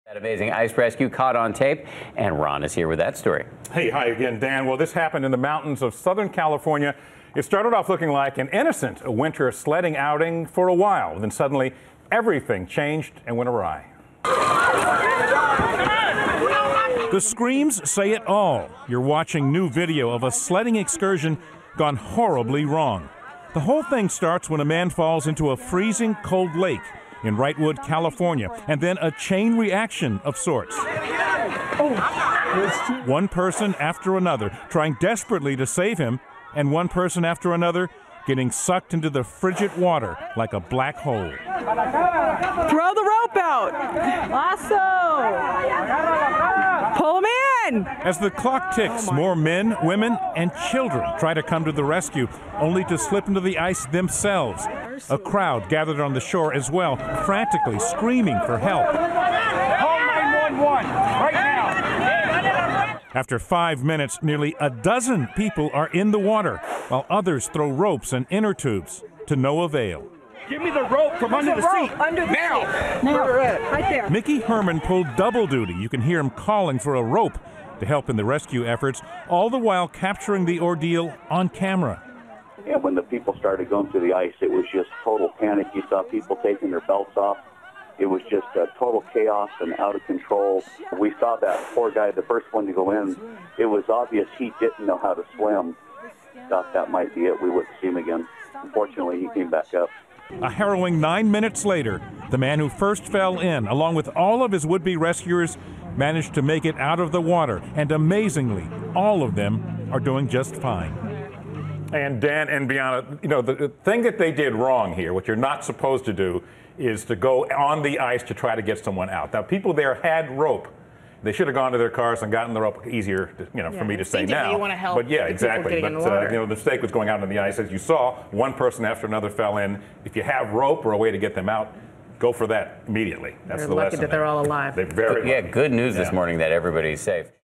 Sledding Accident Rescue Caught on Tape
Ron Claiborne has the latest on those saved after ice breaks on pond.